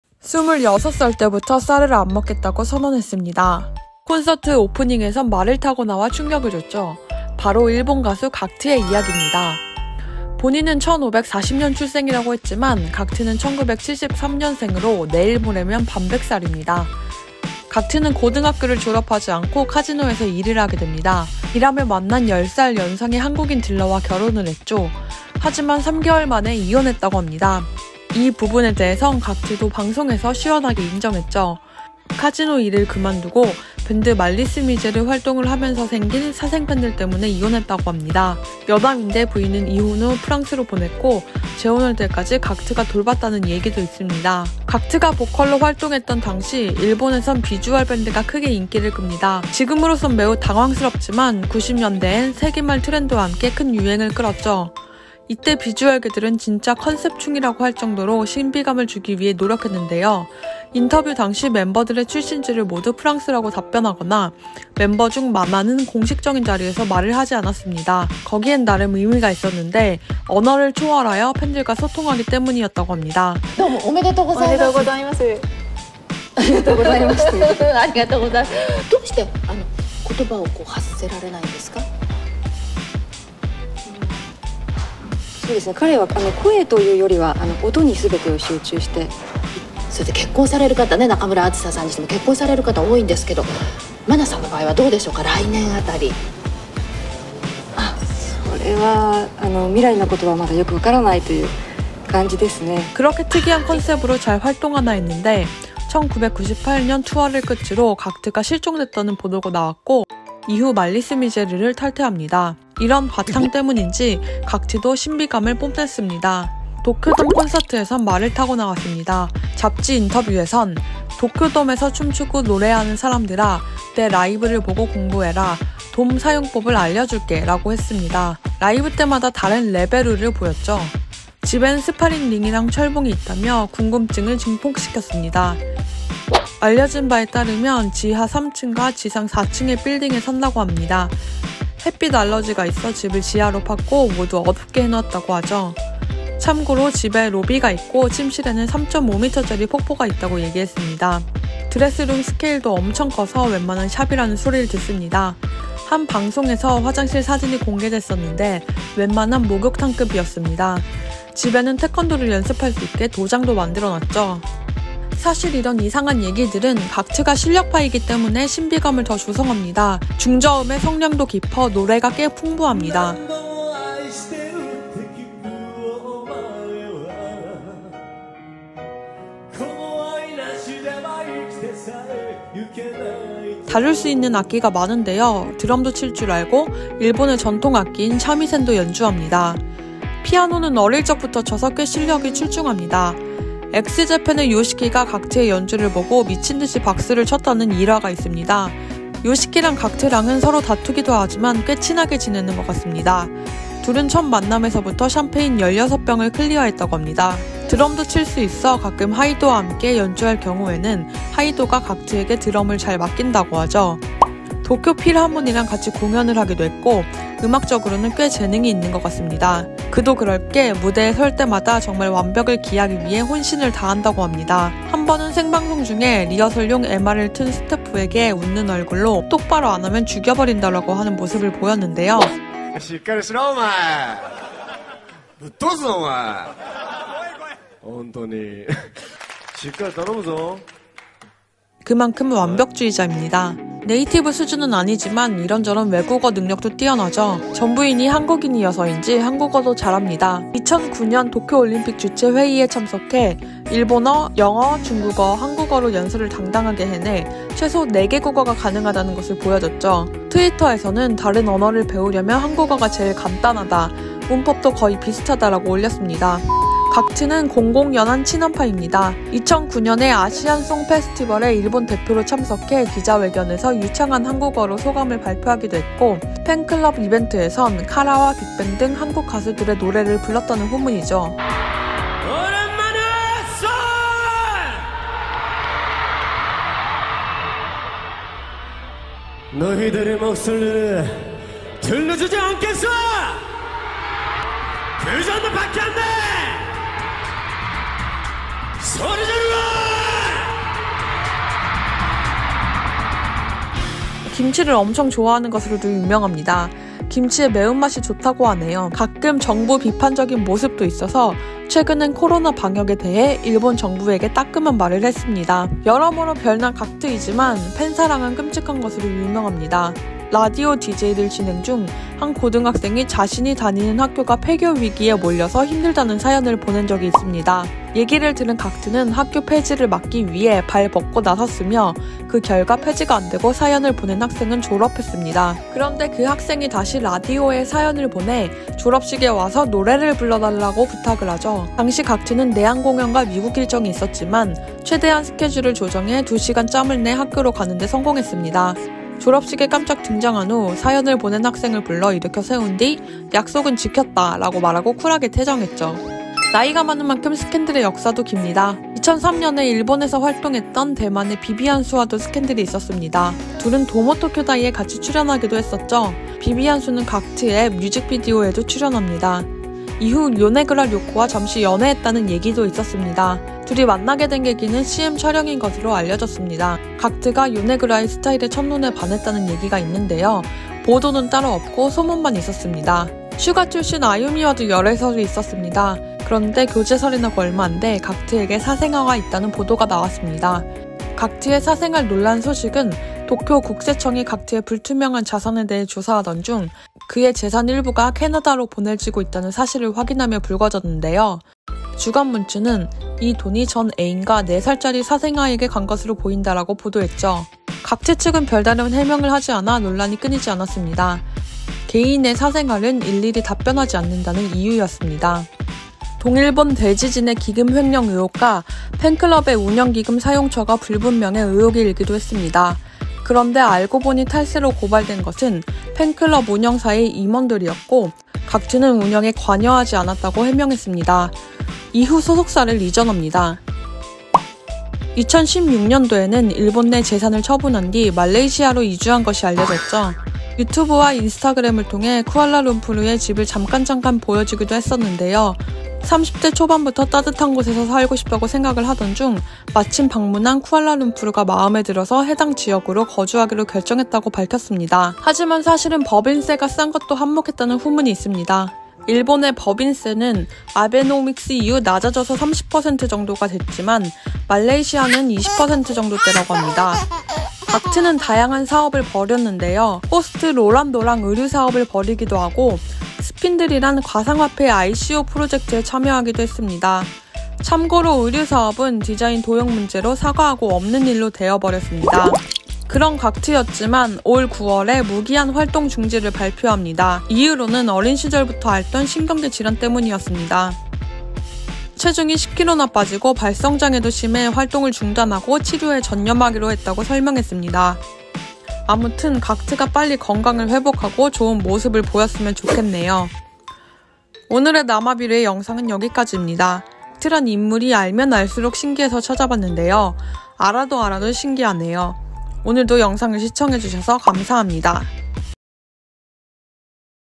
데뷔전부터 현재까지 각트의 인생모음, 엄청난 각트 저택 내부, 떼돈 벌고 성공한 각트의 능력
(댓글은 항상 큰힘이 됩니다!!) 오늘은 가쿠토! 가쿠짱! 각트의 인생에 대해 알아봤습니다. 카지노 딜러 시절부터 현재 활동중단 근황까지 알아봤는데요. 어렸을 때 각트의 인기를 보면서 "왜 저렇게 인기가 많지?" 했지만 알면 알수록 저도 모르게 특유의 신비주의에 빠졌었던 기억이 납니다. 찾다보니 각트도 정말 열심히 살았구나 라는걸 새삼느꼈습니다. 한국에 관심도 많아서 한국인 여자친구도 있었고, 한국어도 배우려고 하죠. 태권도 하는 모습이 인상적이기도 합니다. 어떻게 저렇게 돈을 많이 벌었는지 각트 집에는 폭포도 있고,드레스룸도 엄청 큰걸로 유명하죠. 지금은 말레이시아에 새로운 거주 공간을 만들었다는 근황이네요. 활동중단 후 인스타그램도 비공개로 돌렸지만 팔로신청하면 받아줍니다.ㅋㅋㅋㅋ

여러분 남은 주말 잘 보내시고 모두 한주간 화이팅해요!!